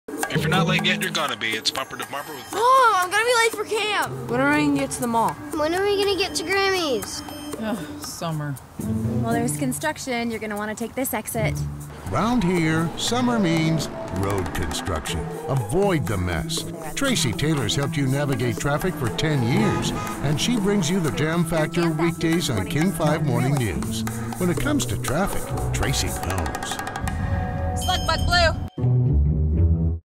Tracy Knows Traffic
Summer in Seattle is all about construction. Count on Tracy Taylor to get you there. Shot with Canon 7d.